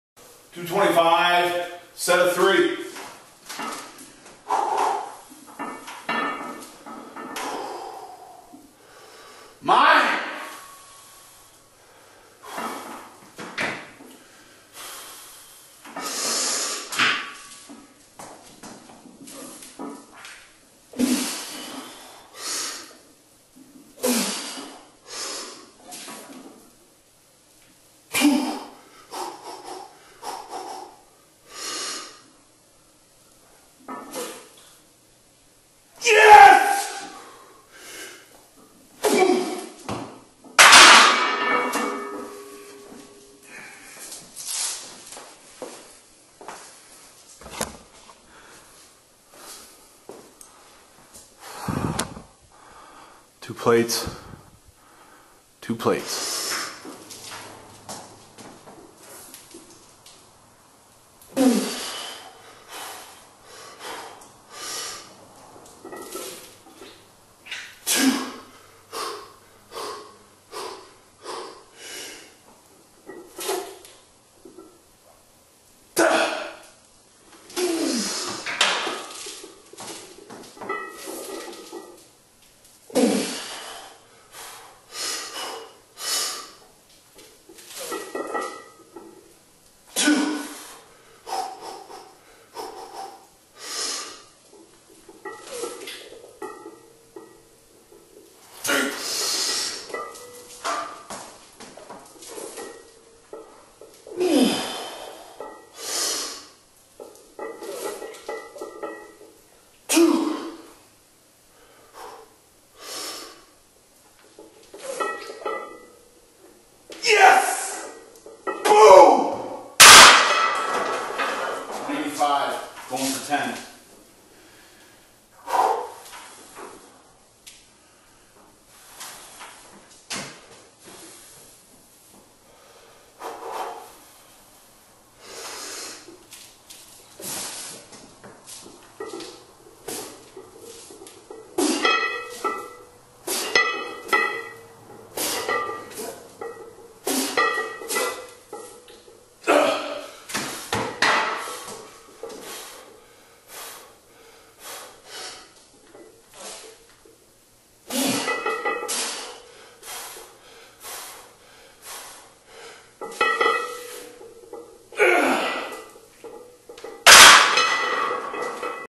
Big Strong Shoulders: Standing Press | 225 x 3 (PR - WOOH!)
Standing Barbell Press from the Rack - All-Time PR: 225 x 3
In the video:  225x3, 215x3, 215x3, 215x3, 185x10